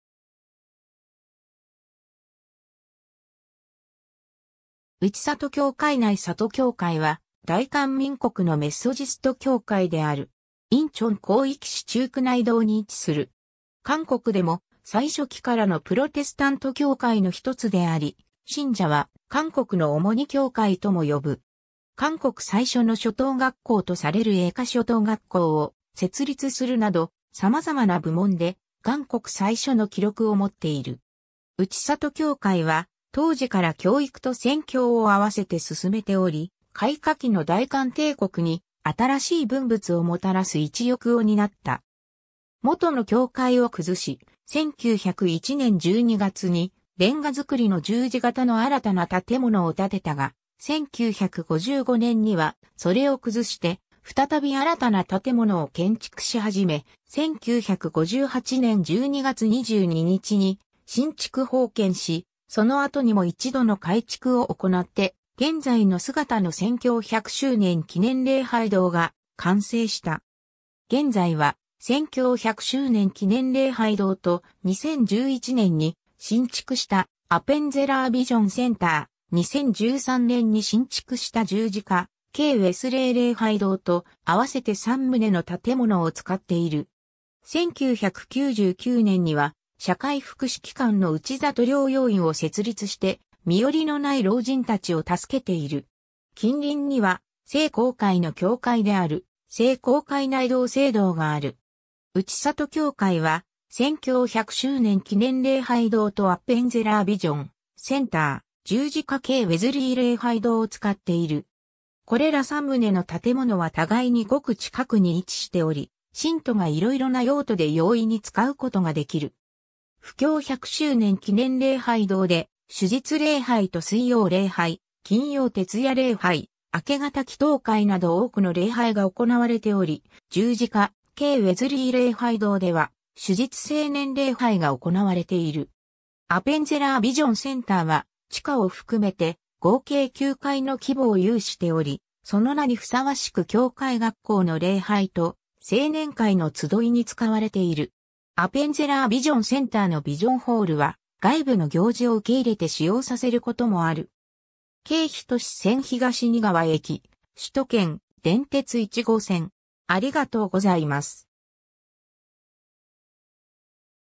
内里教会
中区_(仁川広域市)
内里教会 内里教会（ネリきょうかい、내리교회）は、大韓民国のメソジスト（監理会）教会である。
 仁川広域市中区内洞（ネドン）に位置する。
 韓国でも最初期からのプロテスタント教会のひとつであり、信者は「韓国のオモニ（お母さん）教会（한국의 어머니교회）」とも呼ぶ。
 韓国最初の初等学校（小学校）とされる永化初等学校を設立するなど、様々な部門で「韓国最初」の記録をもっている。
 内里教会は当時から教育と宣教を併せて進めており、開化期の大韓帝国に新しい文物をもたらす一翼を担った。
 元の教会を崩し、1901年12月に煉瓦造りの十字型の新たな建物を建てたが、1955年にはそれを崩して、再び新たな建物を建築し始め、1958年12月22日に新築奉献し、その後にも一度の改築を行なって、現在の姿の宣教100周年記念礼拝堂（선교100주년 기념예배당）が完成した。
 現在は宣教100周年記念礼拝堂と、2011年に新築したアペンゼラー・ビジョン・センター（아펜젤러 비전센터）、2013年に新築した十字架刑ウェスレー礼拝堂（십자가형 웨슬리 예배당）と、合わせて3棟の建物を使っている。
 1999年には社会福祉機関の内里療養院（내리요양원）を設立して、身寄りのない老人たちを助けている。
 近隣には、聖公会の教会である聖公会内洞聖堂がある。
 内里教会は、宣教100周年記念礼拝堂とアペンゼラー・ビジョン・センター、十字架刑ウェズリー礼拝堂を使っている。
 これら3棟の建物は互いにごく近くに位置しており信徒が色々な用途で容易に使うことができる。
 布教100周年記念礼拝堂で主日礼拝と水曜礼拝、金曜徹夜礼拝、明け方祈祷会など多くの礼拝が行なわれており、十字架刑ウェズリー礼拝堂では主日青年礼拝が行なわれている。
アペンゼラー・ビジョン・センターは、地下を含めて合計9階の規模を有しており、その名にふさわしく教会学校の礼拝と青年会の集いに使われている。
アペンゼラー・ビジョン・センターのビジョン・ホールは外部の行事を受け入れて使用させることもある。
 京仁線東仁川駅：首都圏電鉄1号線